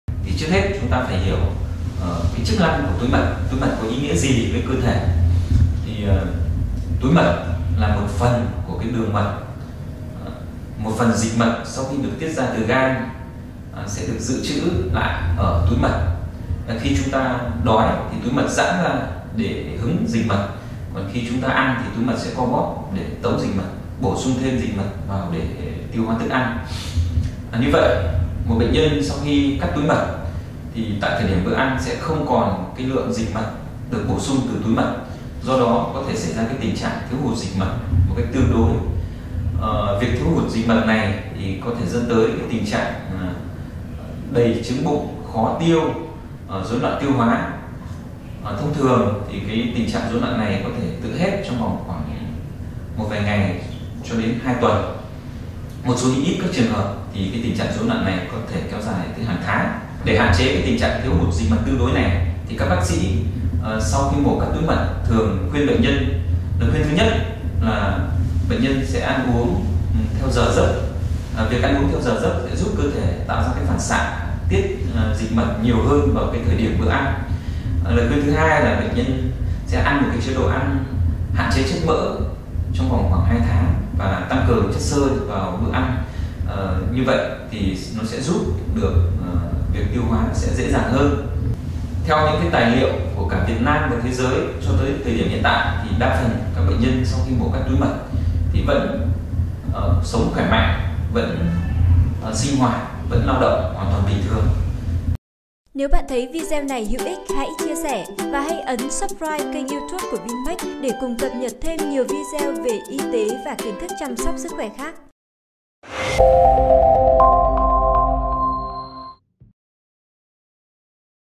Cắt túi mật có ảnh hưởng sức khỏe không? BS Vũ Văn Quân, BV Vinmec Hải Phòng
Bác sỹ Vũ Văn Quân, Khoa Ngoại tiêu hóa – Bệnh viện ĐKQT Vinmec Hải Phòng cho biết: túi mật là một phần của đường mật, Khi đói, túi mật giãn ra để hứng dịch mật, khi ăn thì chúng ta sẽ co bóp để tống dịch mật, bổ sung thêm dịch mật hỗ trợ tiêu hóa thức ăn.

Do đó, sau khi cắt túi mật, bệnh nhân sẽ thiếu hụt dịch mật tương đối, từ đó có thể dẫn tới tình trạng đầy bụng, khó tiêu, rối loạn tiêu hóa… trong vài ngày đến 2 tuần.

Để hạn chế tình trạng thiếu hụt dịch mật tương đối, sau mổ cắt túi mật, bệnh nhân thường được khuyên tuân thủ một số điều sau:

Chế độ ăn uống theo giờ giấc phù hợp để giúp cơ thể tạo phản xạ tiết dịch mật nhiều hơn vào giờ ăn
Hạn chế chất mỡ trong khoảng 2 tháng và tăng cường chất xơ vào mỗi bữa ăn.

Theo các nghiên cứu trong và ngoài nước, đa phần bệnh nhân sau khi mổ cắt túi mật vẫn sống khỏe mạnh.